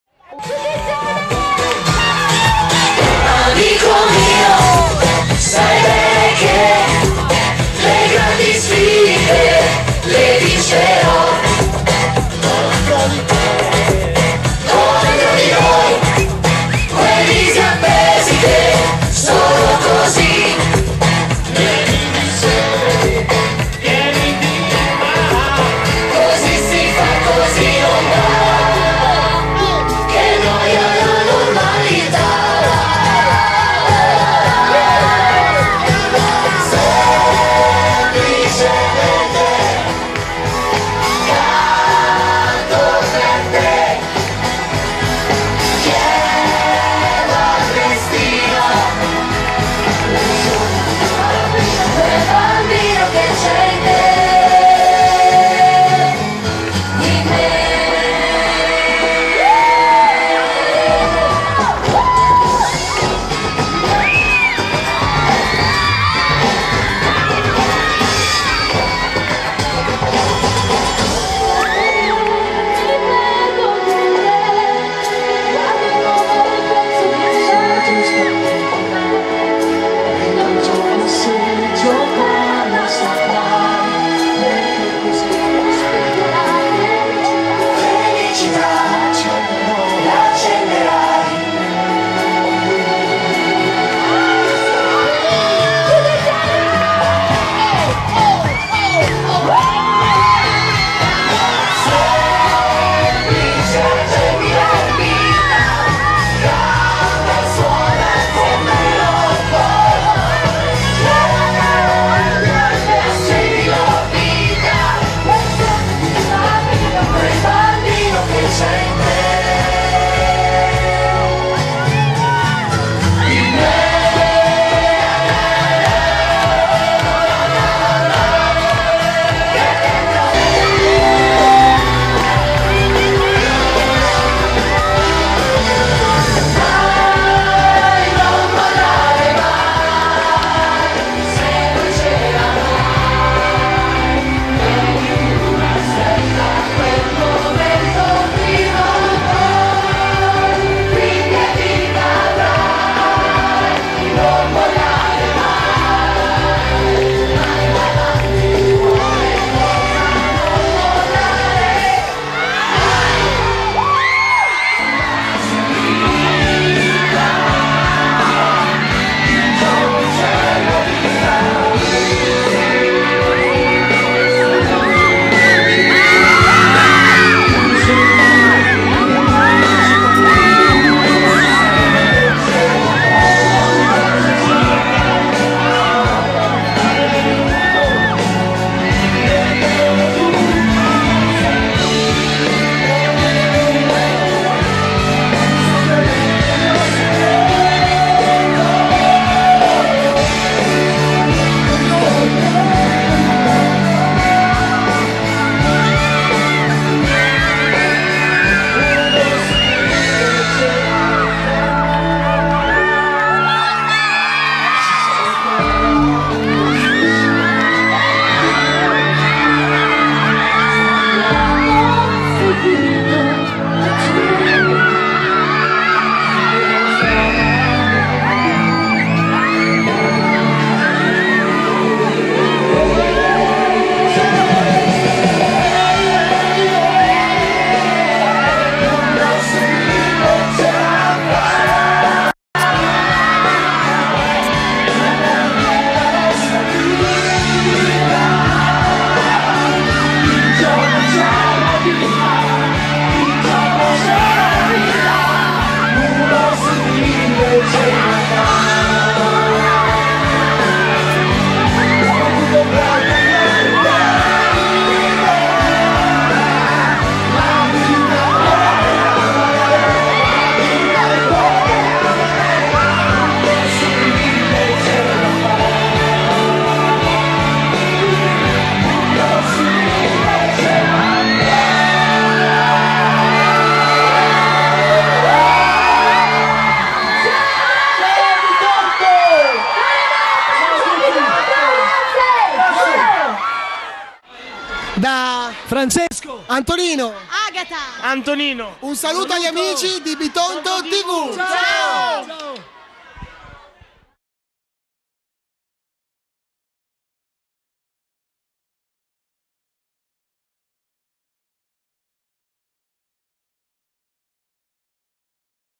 BITONTO - Gli Amici di Maria De Filippi
1 settembre 2008 - concerto dal vivo del team di ragazzi del cast di "Amici"